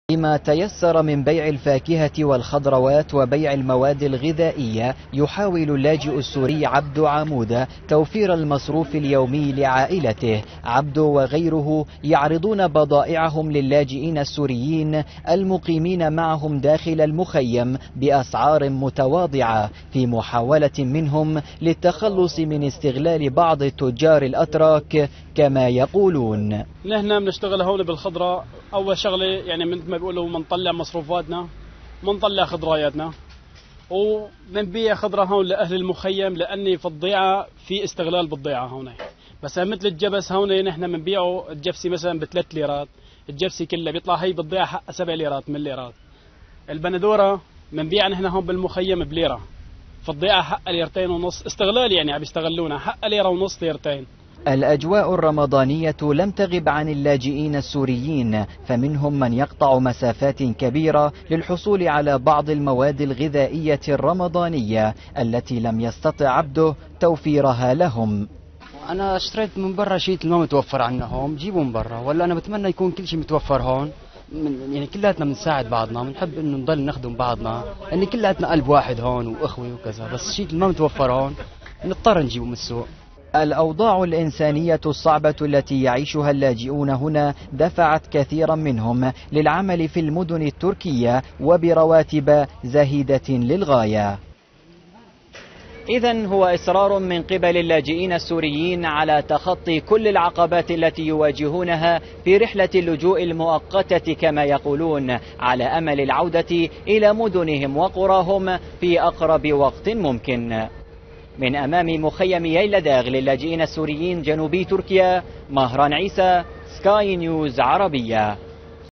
الأوضاع المعيشية للاجئين السوريين بتركيا
يعاني اللاجئون السوريون في مخيم يايلاداغ بتركيا من ظروف معيشية صعبة تدفعهم للبحث عن عمل بعيدا عن المخيم بمقابل نقدي بخس، ويشتكي آخرون من قلة المواد الغذائية في المخيم. لمشاهدة المزيد من الفيديوهات يرجى الضغط على الرابط التالي